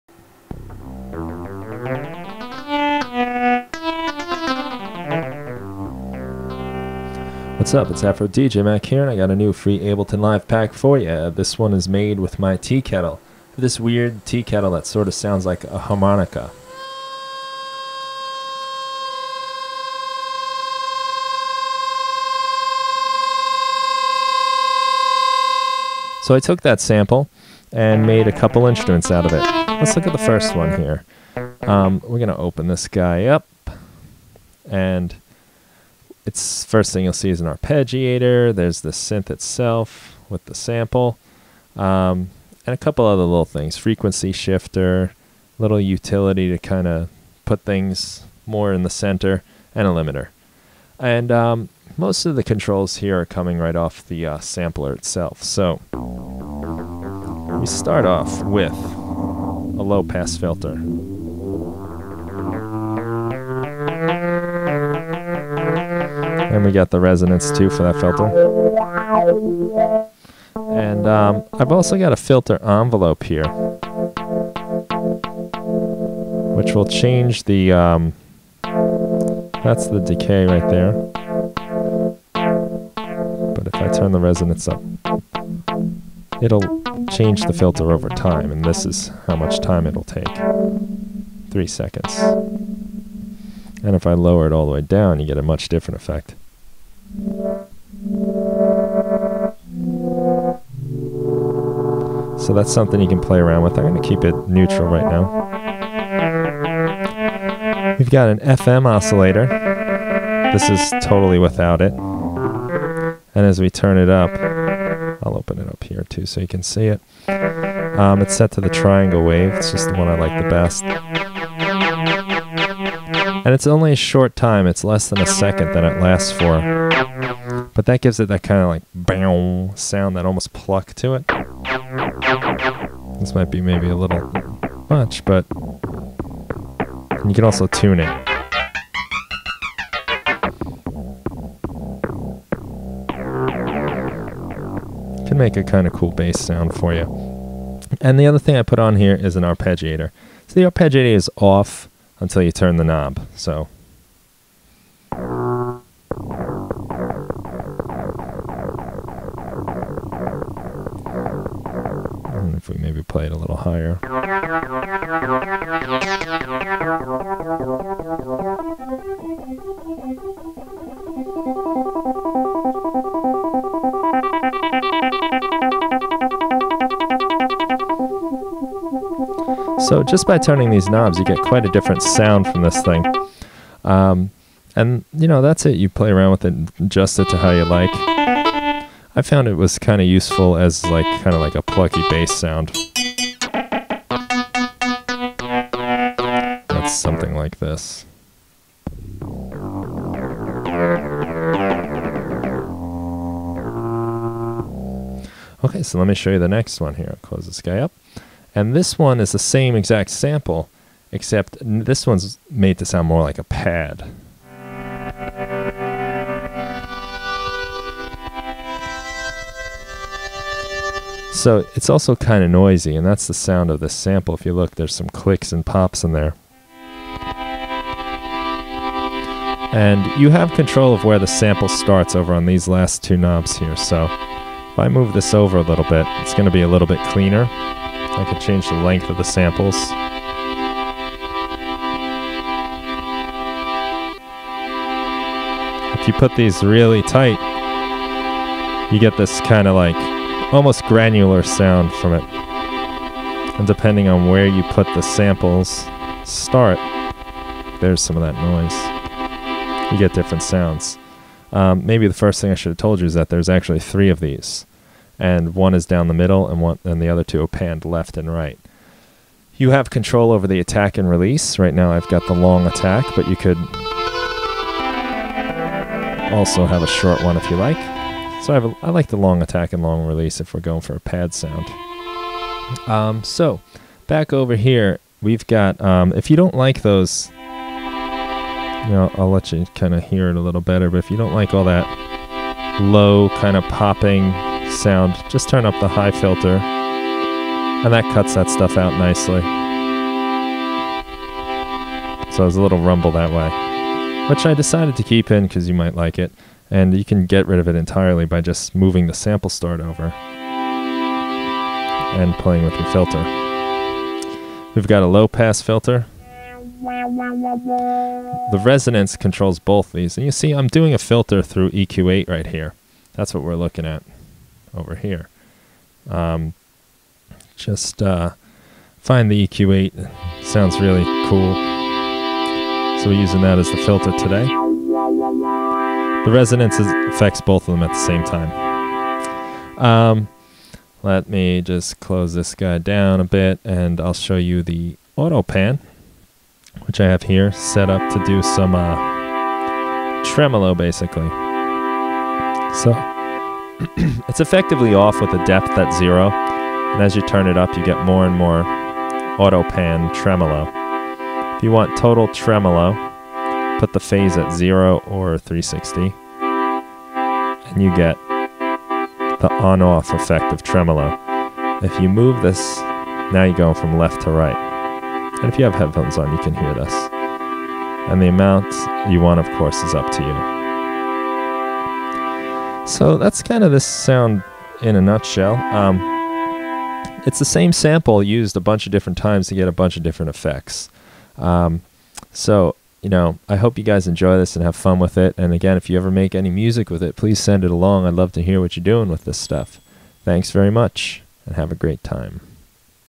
Tea Kettle Synths for Ableton Live
Free Ableton Live Synths made with samples of a tea kettle. Get em while they're hot.